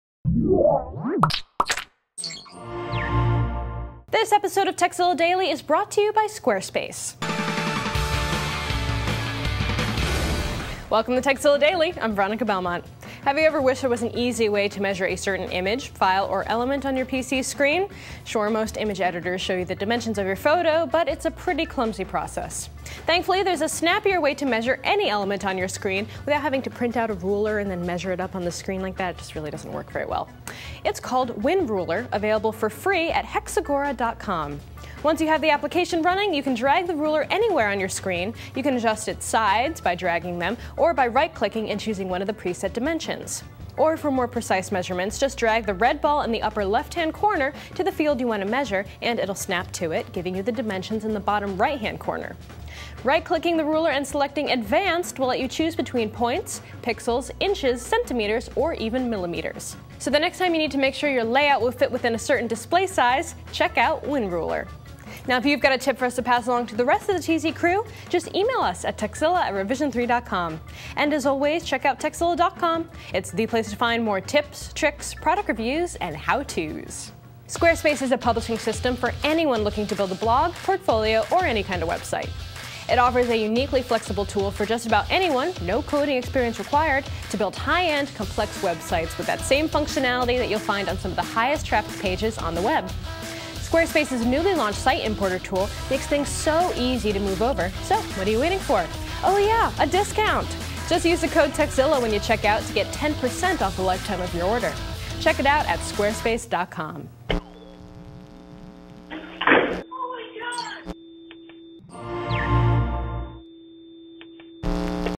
A Virtual RULER Build Right Into Your Computer Screen! - Tekzilla Daily Tip
Use a virtual ruler to calculate the size of your windows.